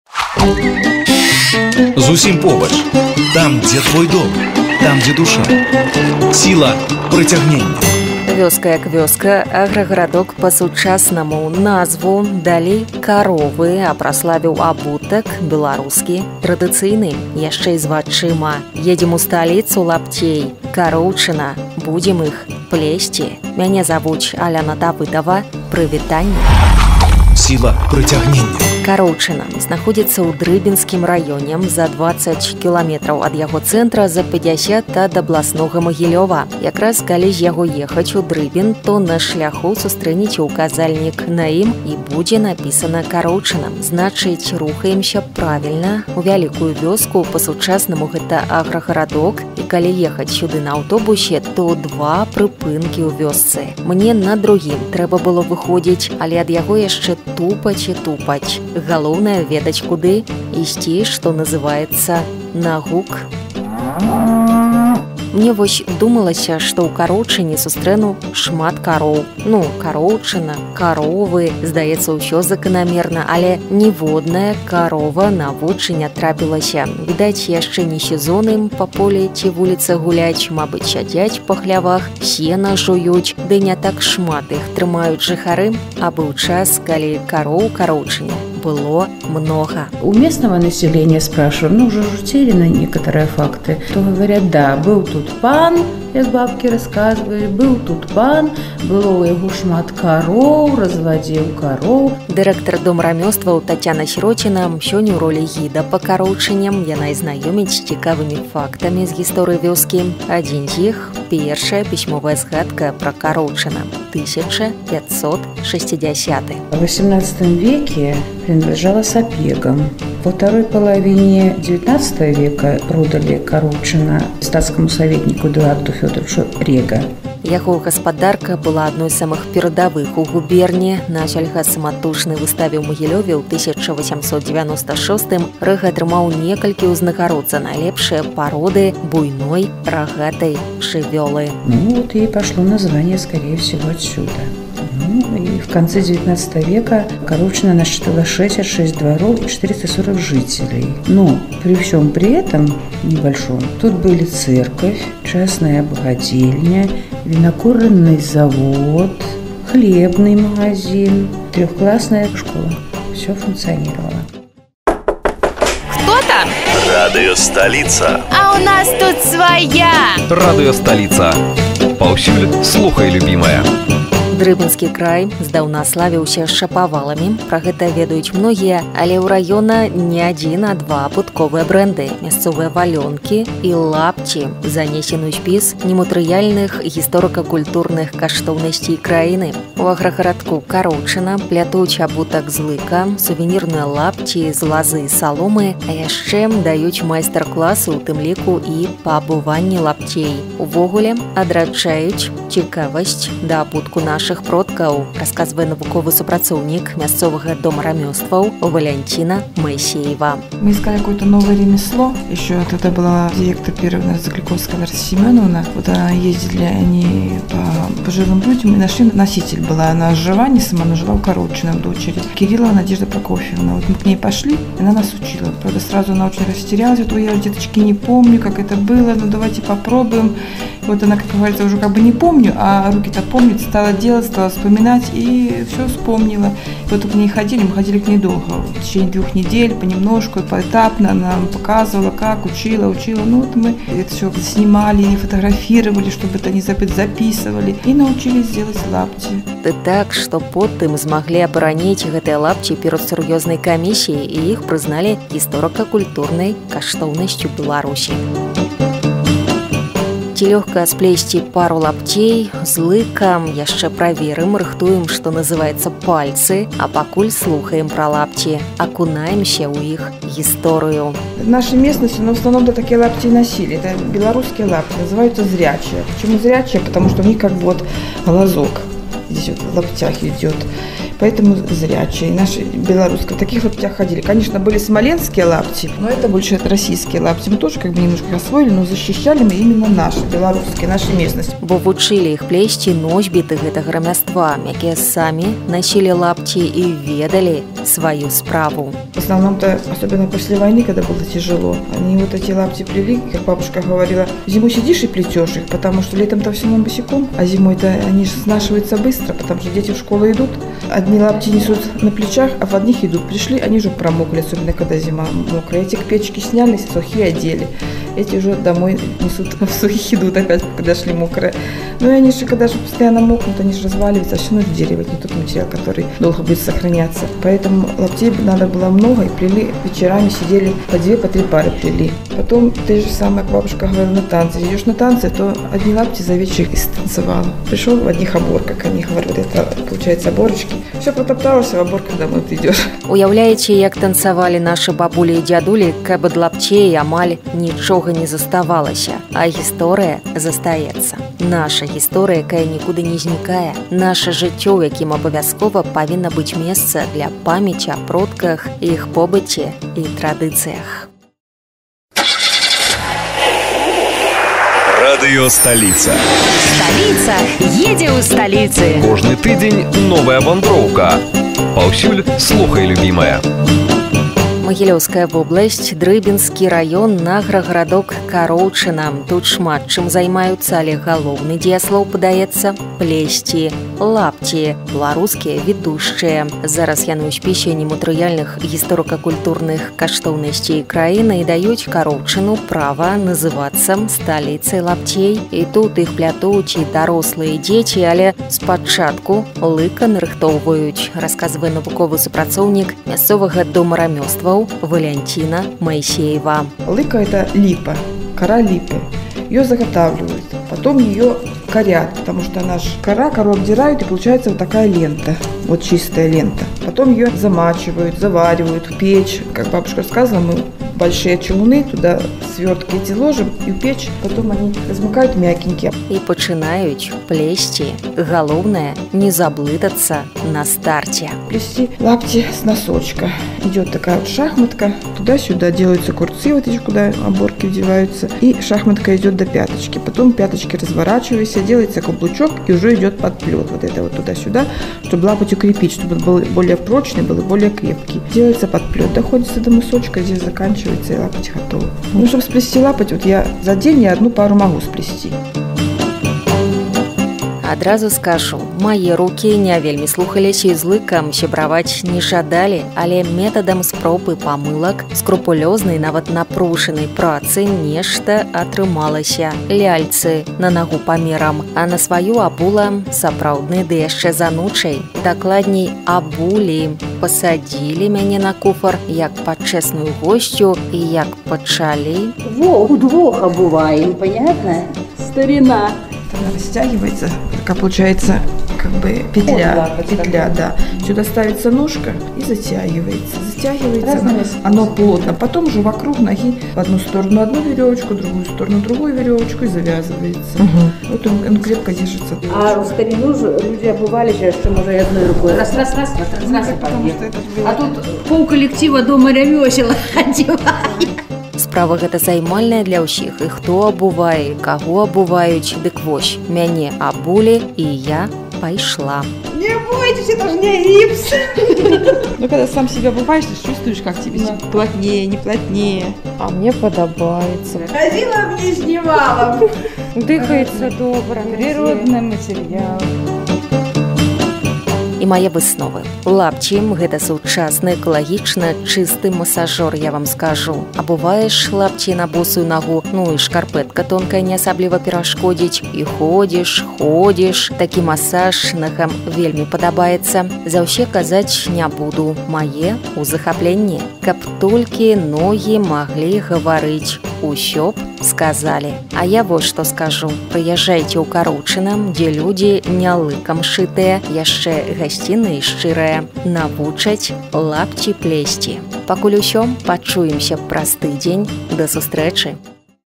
Столица лаптей | Коровчино | СИЛА ПРИТЯЖЕНИЯ
КОРОВЧИНО. 20 км - от районного центра, 50 – от областного Могилева. Название деревне дали коровы. А прославили - лапти, белорусские традиционные. Ещё и с глазами. Они - историко-культурная ценность Беларуси.
ТАЙМКОДЫ:
00:00 Знакомство со столицей лаптей
00:49 Идём на звук
01:08 Коровы в Коровчино
01:50 Факты из биографии
03:00 Обувь - историко-культурная ценность
04:20 Особенности "зрячих" лаптей
06:37 Заготвливаем лыко
08:39 Что ощущают ноги в лаптях?